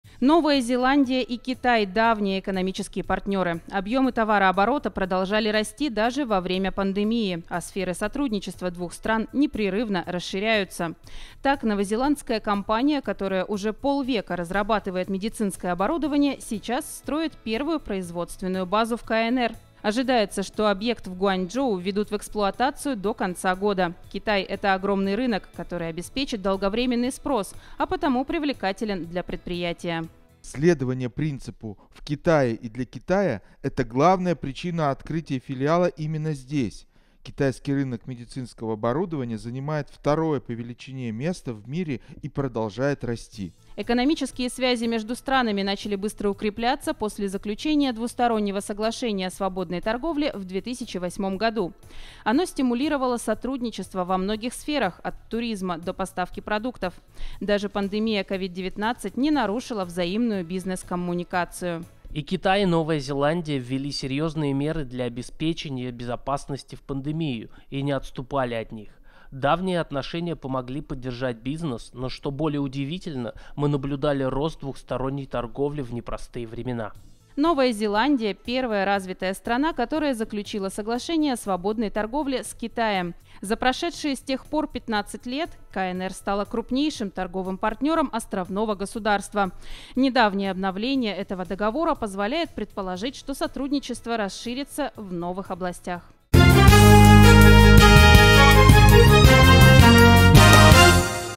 Медицинская компания из Новой Зеландии строит базу в Китае
Новая Зеландия и Китай – давние экономические партнёры. Объёмы товарооборота продолжали расти даже во время пандемии, а сферы сотрудничества двух стран непрерывно расширяются. Так, новозеландская компания, которая уже полвека разрабатывает медицинское оборудование, сейчас строит первую производственную базу в КНР. Ожидается, что объект в Гуанчжоу введут в эксплуатацию до конца года. Китай – это огромный рынок, который обеспечит долговременный спрос, а потому привлекателен для предприятия.